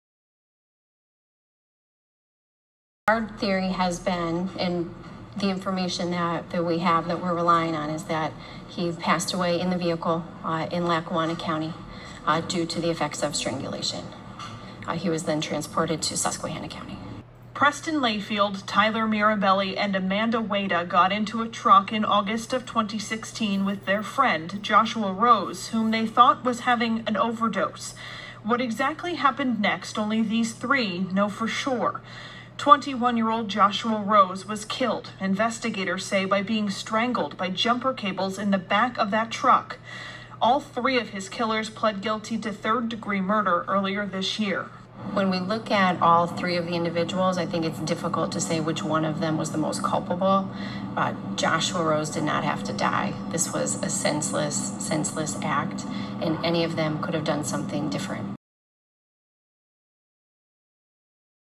Experience Matters: Case of Joshua Rose
Callous. Cruel. Bizarre. Senseless. -- Those words only begin to touch the horror and heartbreak that came with the case of Joshua Rose.

As Chief Deputy District Attorney, I was there through every step of the investigation. I was on scene when Joshua's body was found in the woods and I attended his autopsy as we pieced together what had happened to him. Despite the disturbing details of the case, I never wavered in my pursuit of justice for Joshua and his family. It was an honor to be part of the team that secured justice for this young man whose life was tragically cut short.

As I seek to become Magisterial District Judge, I draw on my experience with cases like Joshua's, and I pledge to continue to serve with the same dedication, compassion, and unwavering commitment to justice.